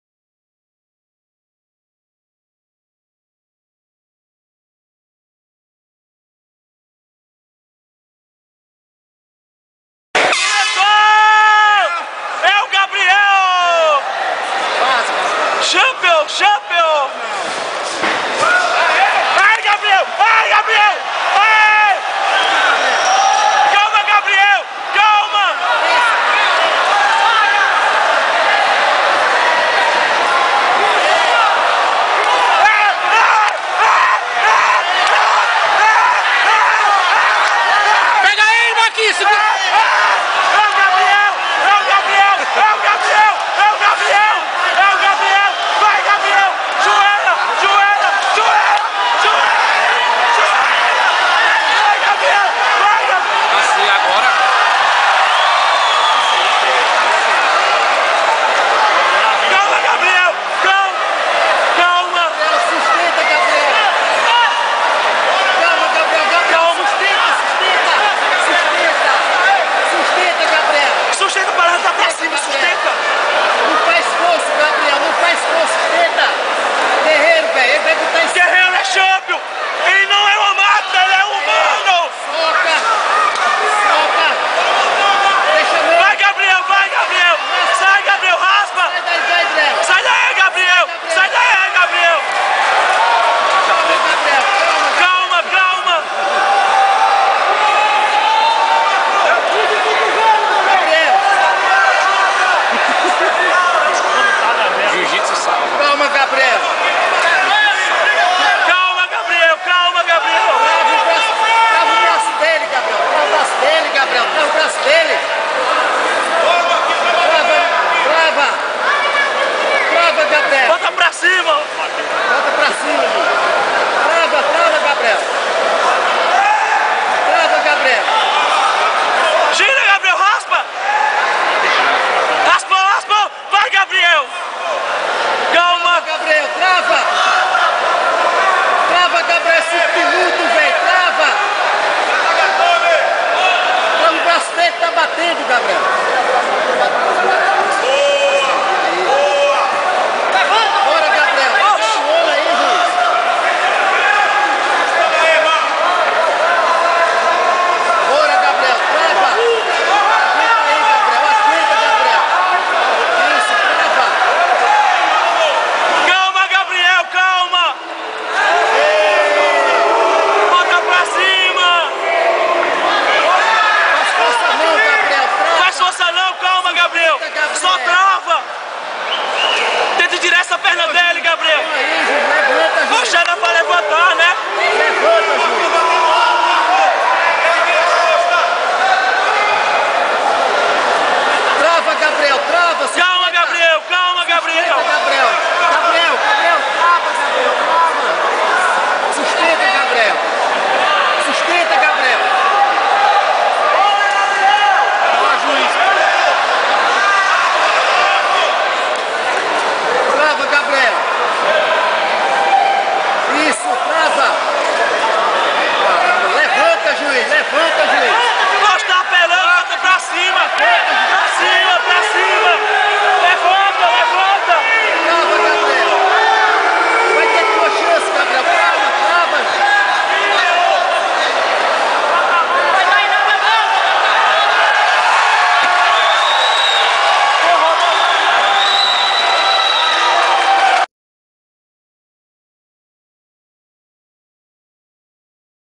GFC 9ª Edição - Marcos Vinicius X Gabriel
Luta realizada dia 19 de janeiro de 2013 em Vitória-PE, Vitória de Marcos Vinicius por finalização.